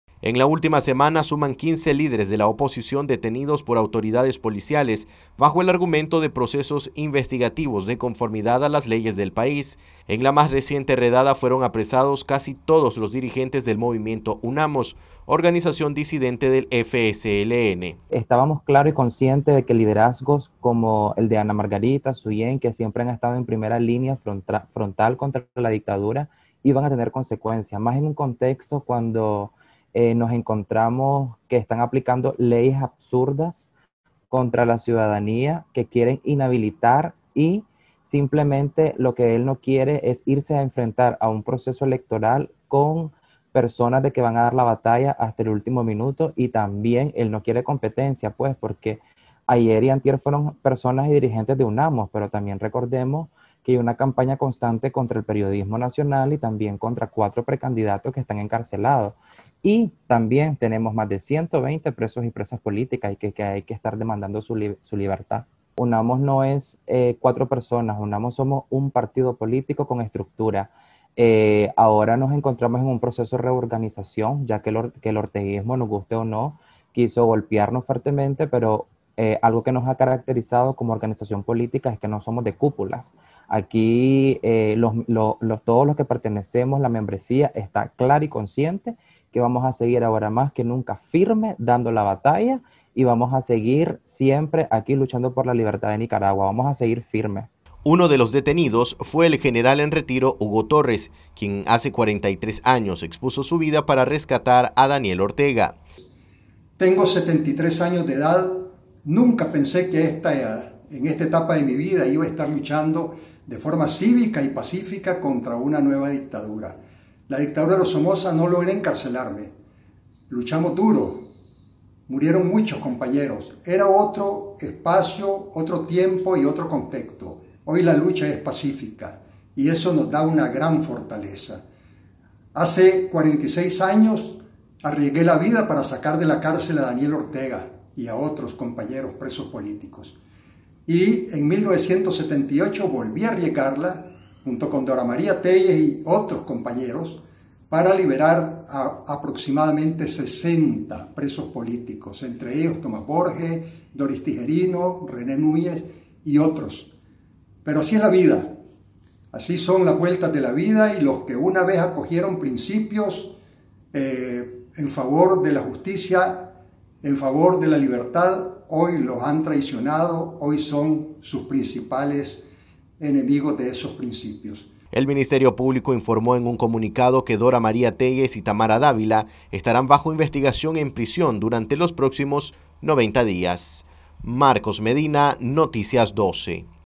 90 días de detención para dirigentes del Movimiento UNAMOS, informa Ministerio Público
NosResistimosAlEmbargo La mayoría de dirigentes del Movimiento UNAMOS están detenidos por presunta traición a la patria.